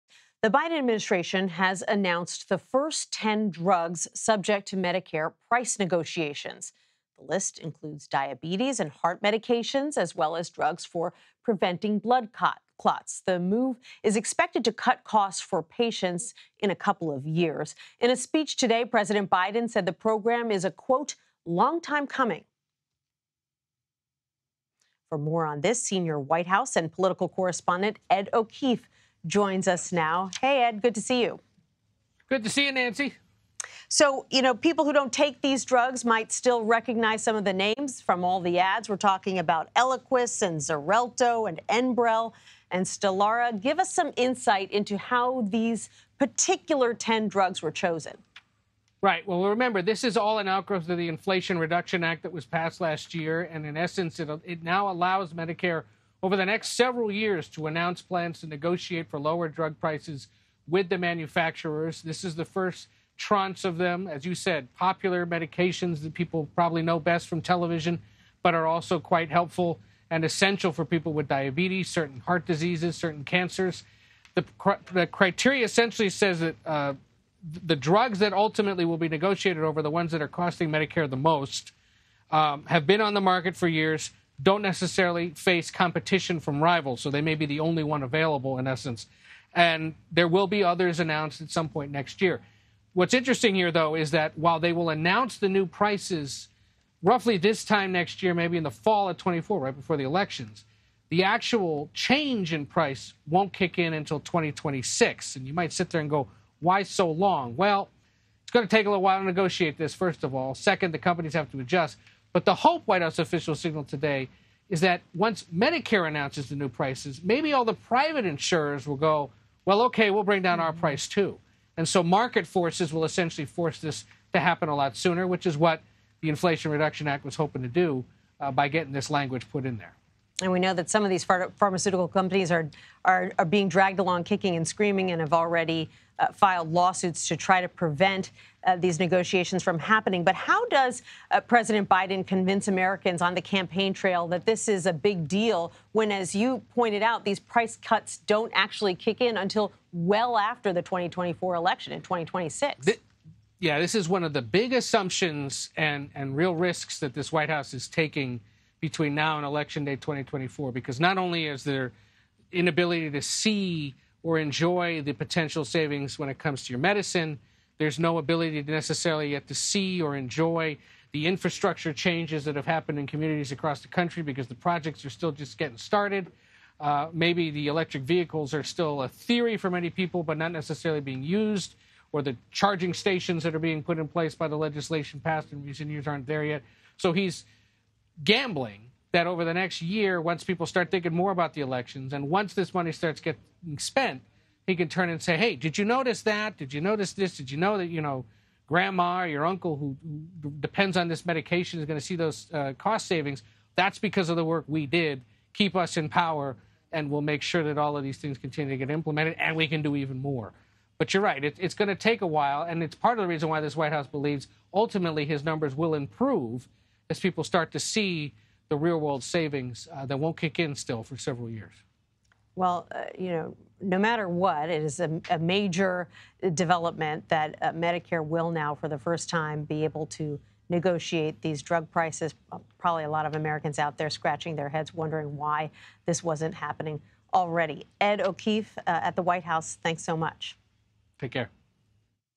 Drug price cuts won't kick in for 3 years. Can they still help Biden in 2024?
The Biden administration on Tuesday announced the first 10 drugs subject to Medicare price negotiations. The move is expected to cut costs for patients, but the discounts won't kick in until 2026. CBS News senior White House and political correspondent Ed O'Keefe reports.